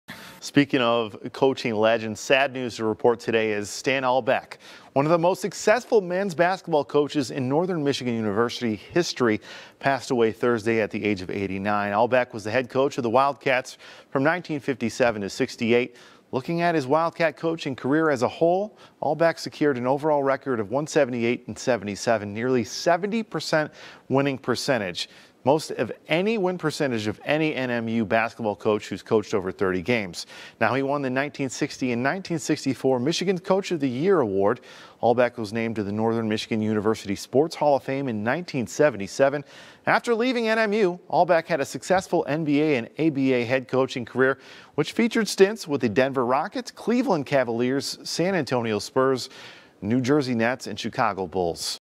Legendary NMU Basketball Coach Stan Albeck Passes Away
Sad news to report today as Stan Albeck, one of the most successful men's basketball coaches in Northern Michigan University history, passed away Thursday at the age of 89.